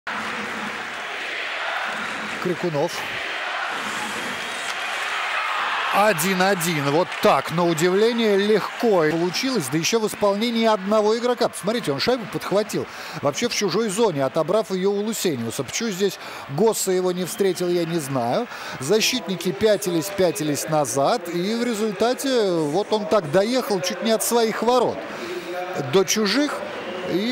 Гол Ильи Крикунова / Krikunov's self-made goal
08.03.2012, Илья Крикунов все сделал сам